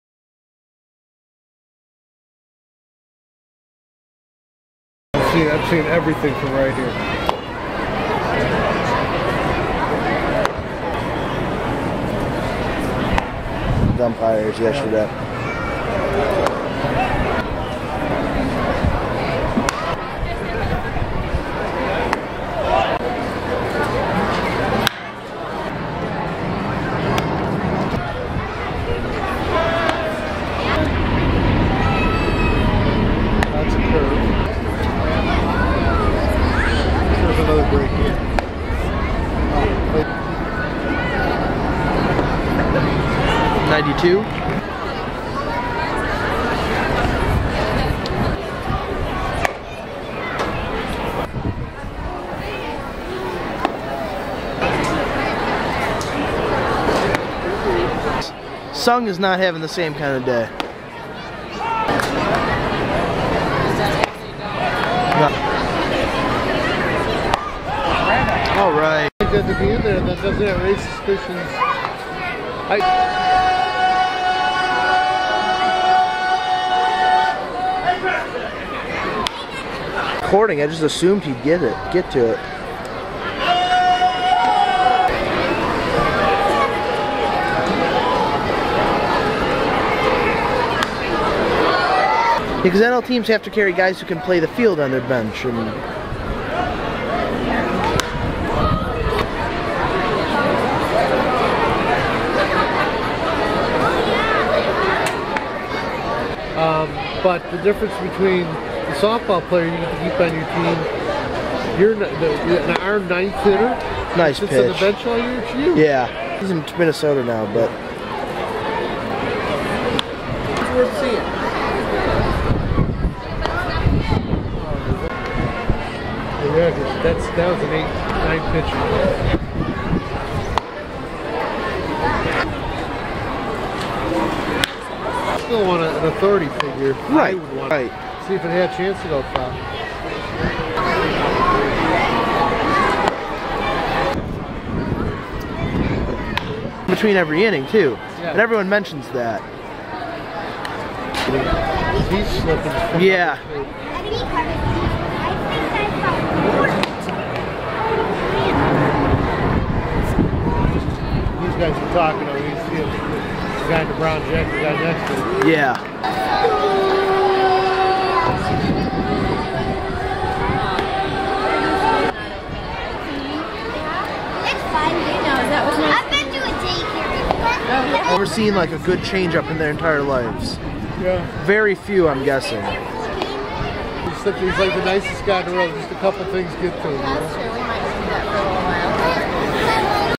Jen-Ho Tseng, RHP, Chicago Cubs
Video of Chicago Cubs RHP prospect Jen-Ho Tseng